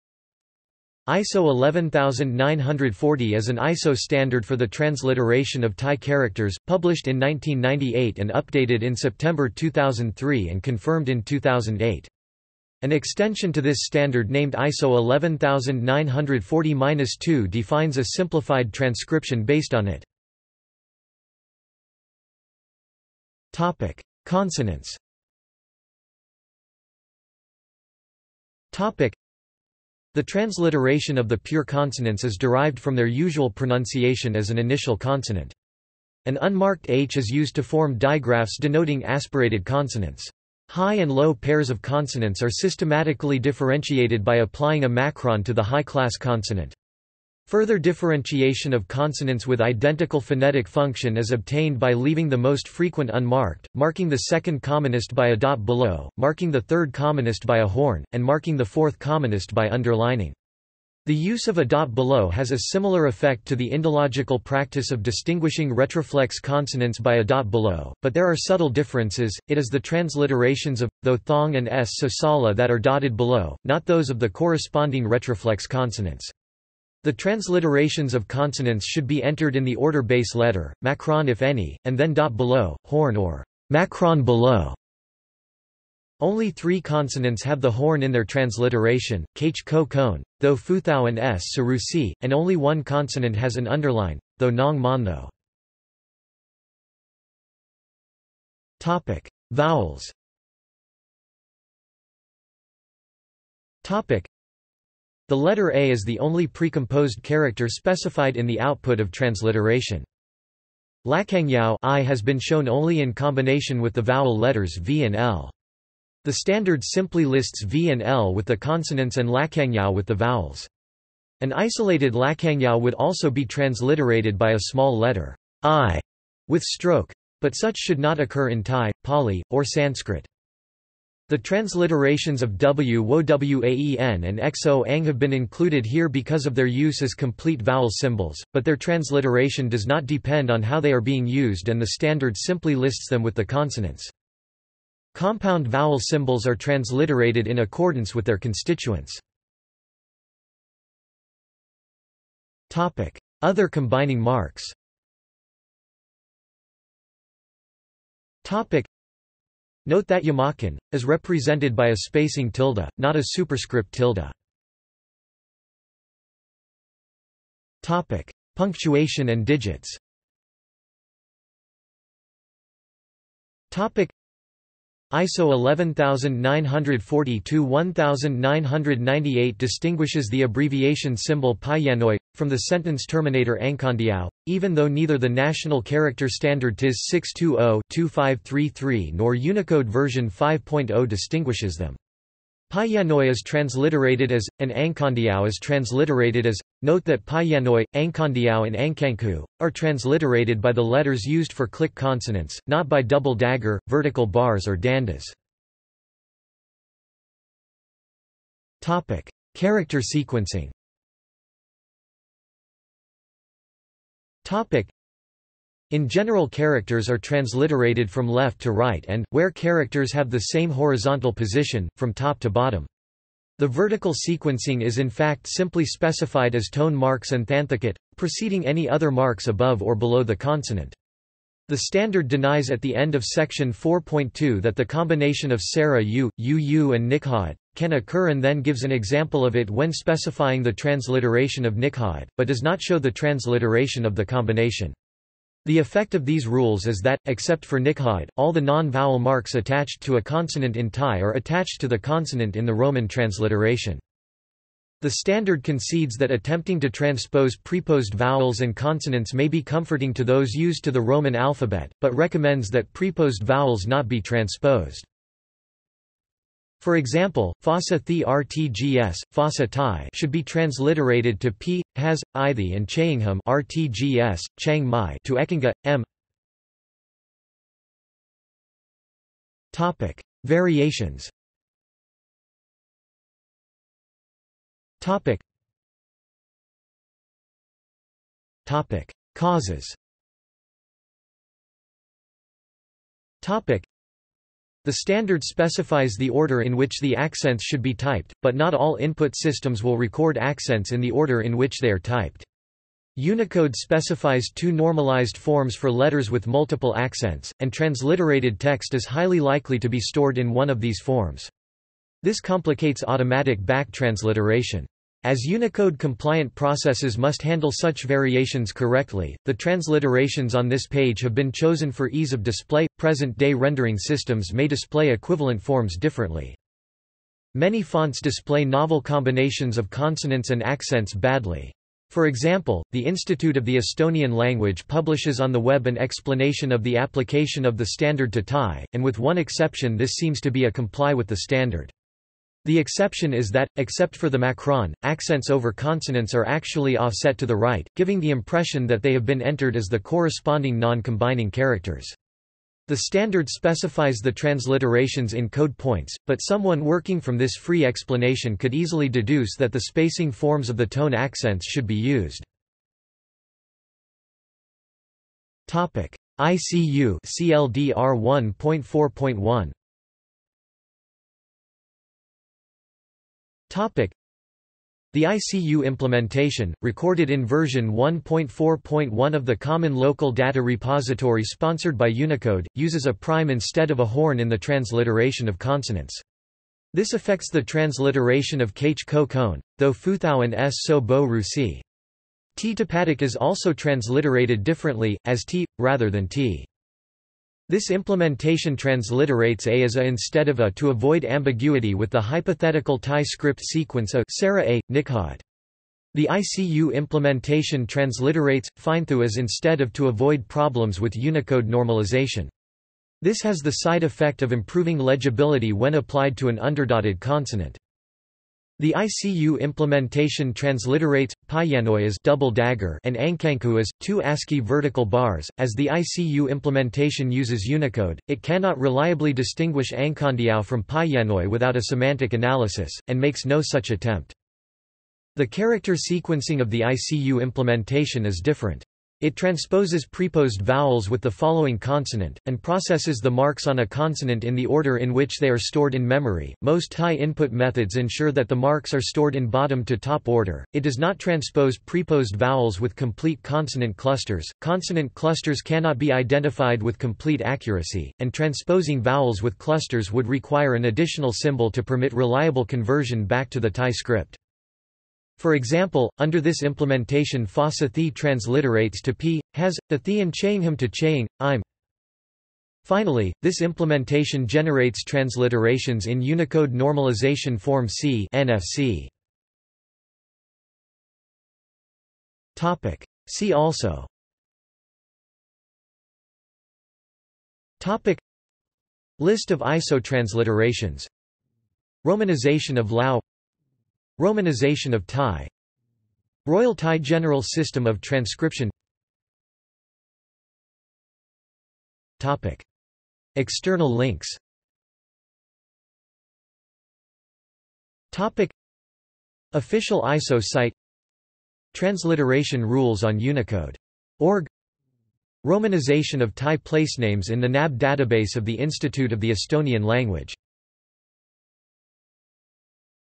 ISO 11940 | Wikipedia audio article
This is an audio version of the Wikipedia Article:
ISO 11940

SUMMARY
ISO 11940 is an ISO standard for the transliteration of Thai characters, published in 1998 and updated in September 2003 and confirmed in 2008. An extension to this standard named ISO 11940-2 defines a simplified transcription based on it.